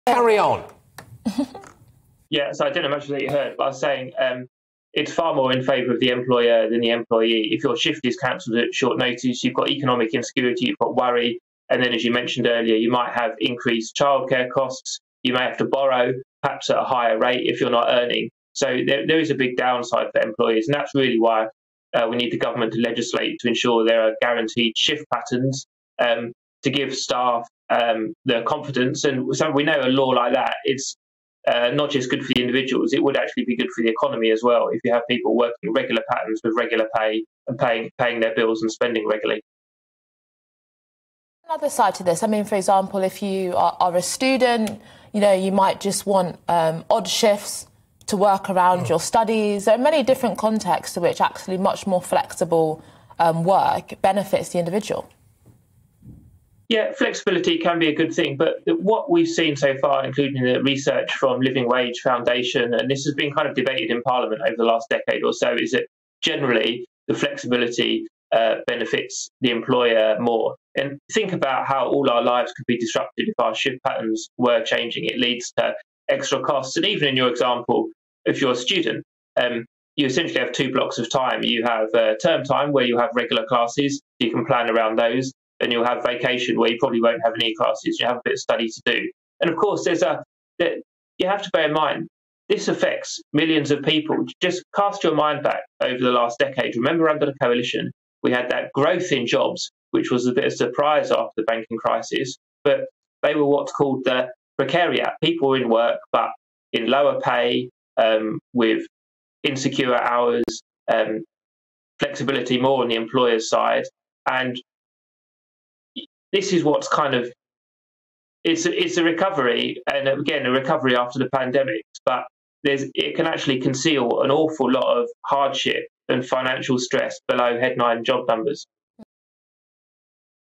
Peter Edwards: Less than a week's notice of shifts is 'in favour of the employer'
‘It’s far more in favour of the employer than the employee’

Peter Edwards calls on the Government to address the issue of one in three workers getting less than a week’s notice for shifts.